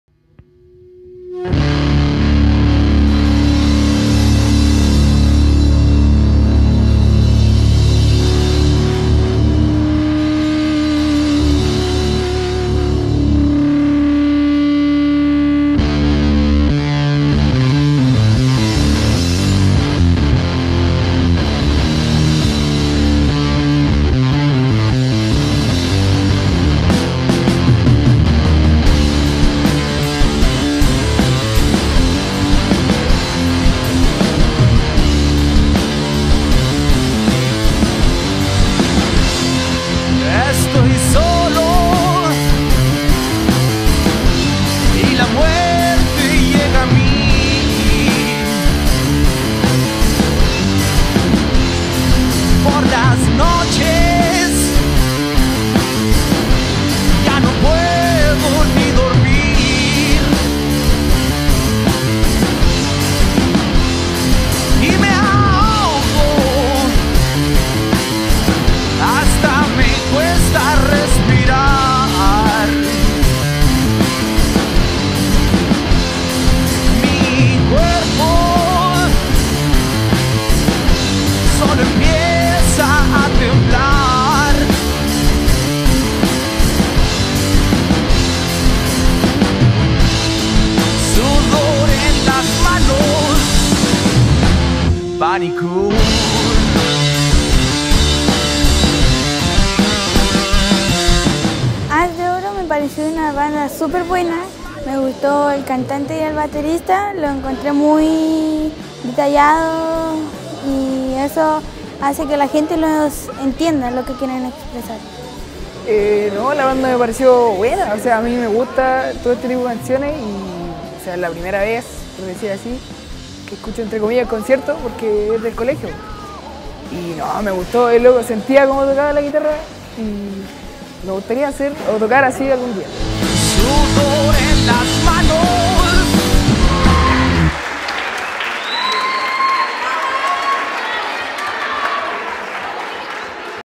Apreciación de la Música Nacional: As de Oros en Puerto Williams
En el marco del Programa de Apreciación de la Música Nacional en el Contexto Escolar, la banda de Rock Blues As de Oros visita el Liceo Donald Mc Intyre Griffiths revisa parte de lo que sucedió.